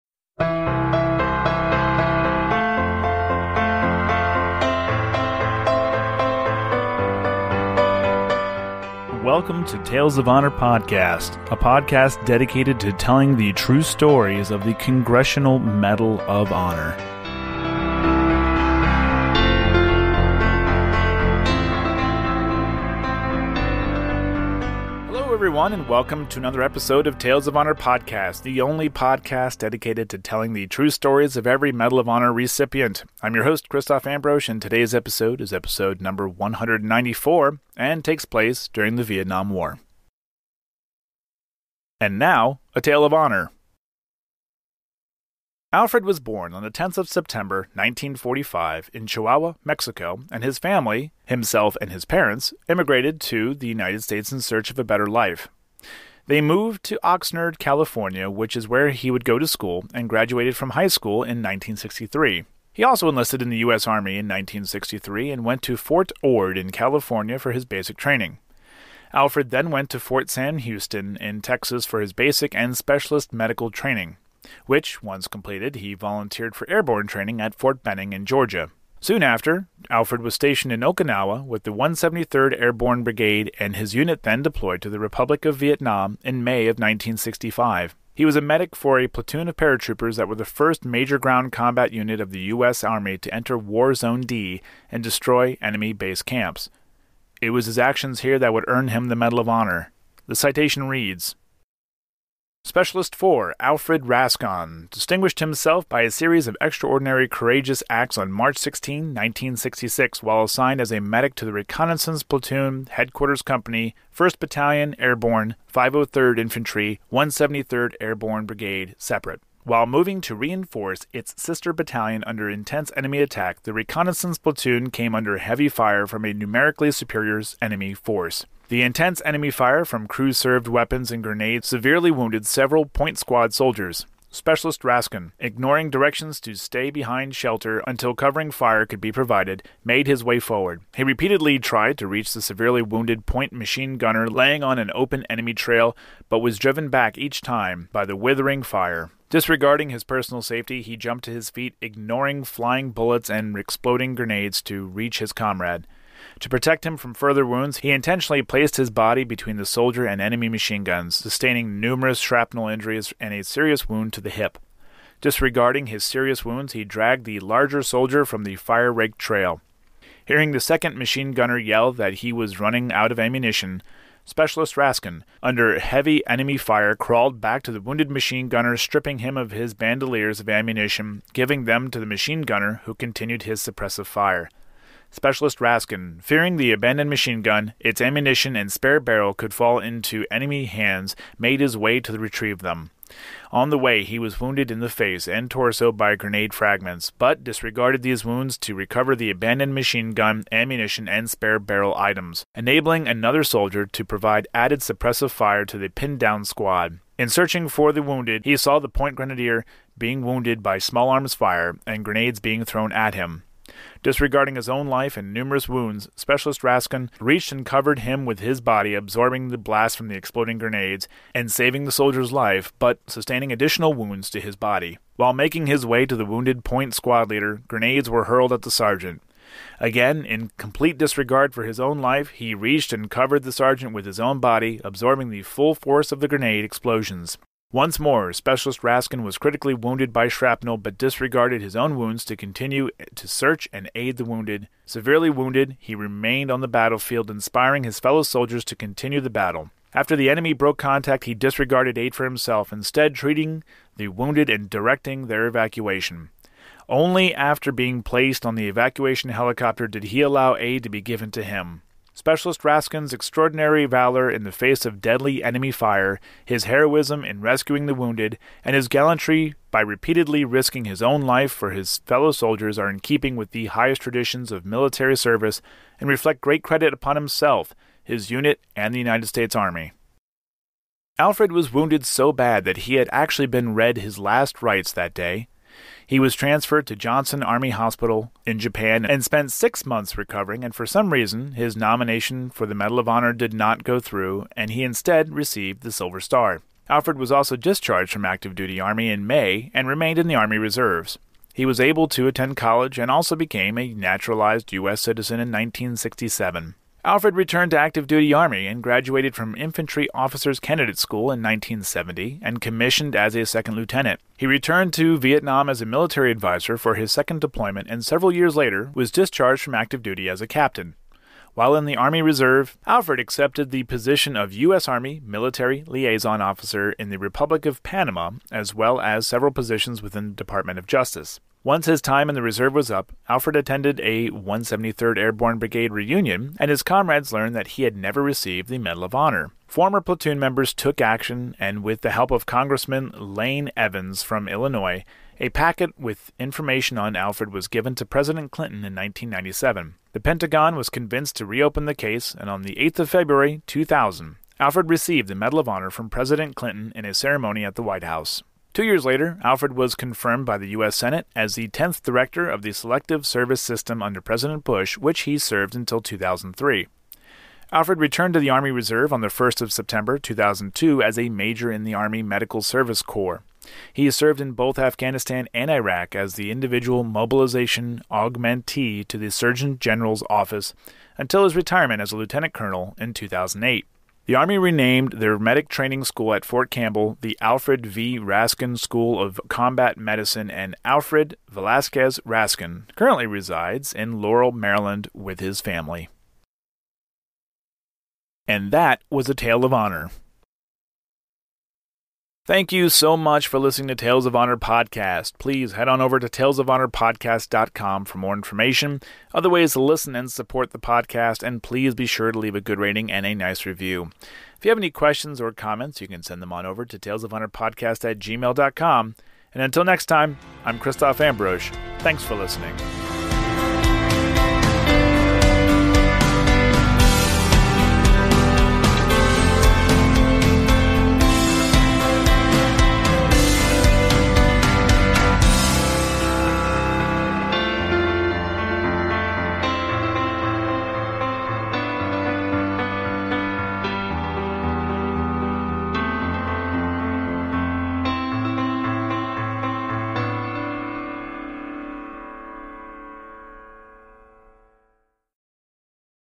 Alfred V Rascon - Medal of Honor Recipient
On episode one hundred and ninety-four, the story of Alfred V Rascon is told.

 All stories in April and May will be from the Vietnam War.